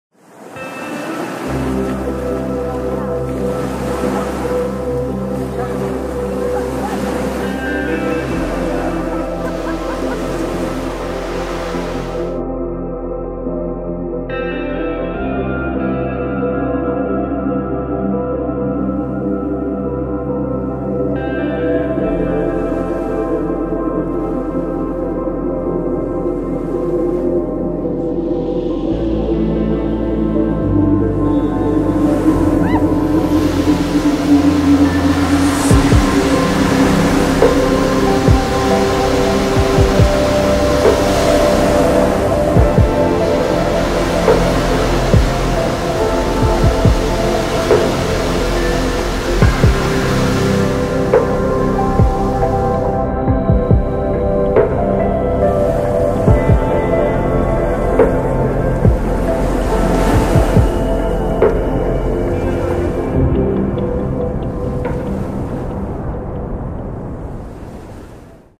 The Old Alpha Beach Cinematic - Sony A7sii Canon 50mm f1.8 STM
Short Cinematic video of the
Old Alpha beach Lagos Nigeria.